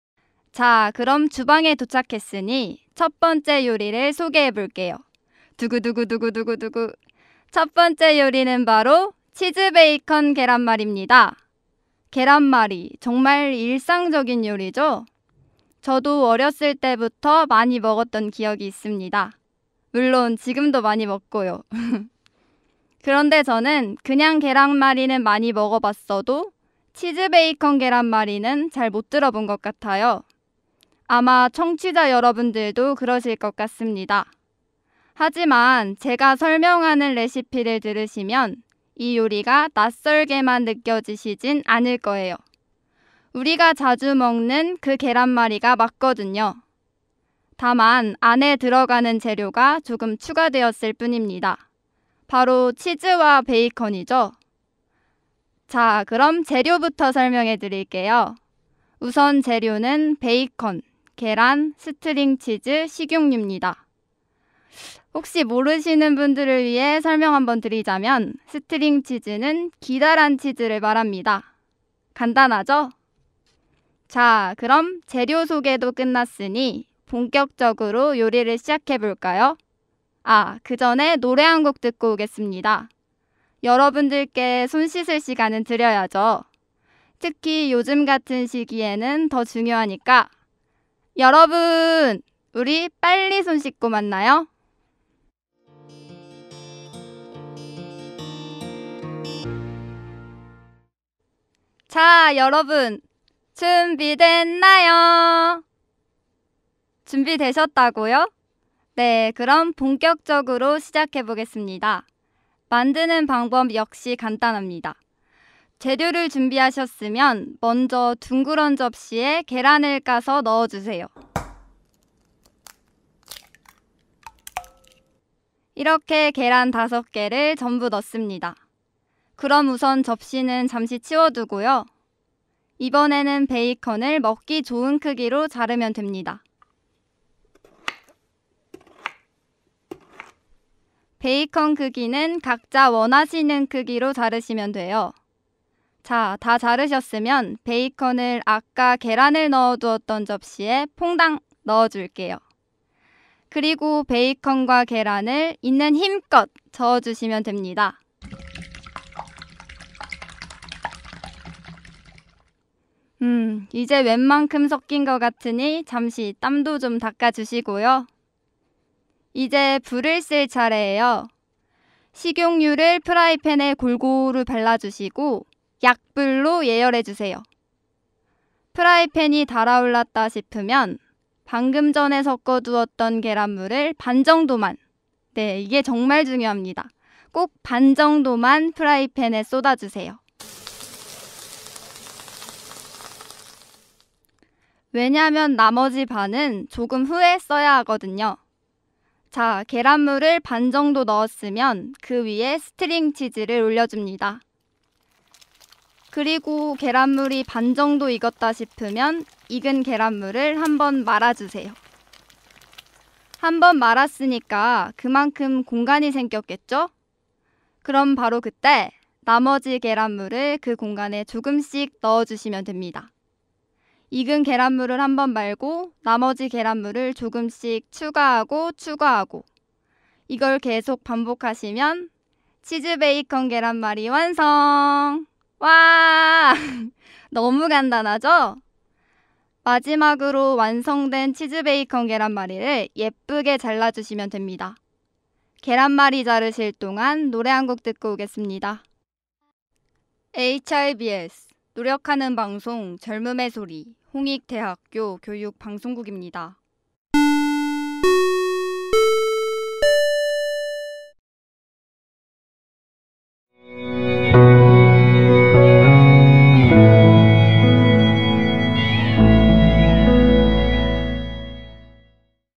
🌈컬러풀 썰스데이 - 2주차 : 얼렁뚱땅 요리비책
벌써 1년 가까이 지속되고 있는 코로나!!
이로인해 외식이 매우 어려워졌는데요...
집에서 항상 뻔한 메뉴만 먹느라 지치셨다고요??
그런 여러분들을 위해 준비했습니다!
특색있지만 맛있는 레시피를 모아둔 얼렁뚱땅 요리비책!

PD 권지원
ANN 강예지
ENG 이현영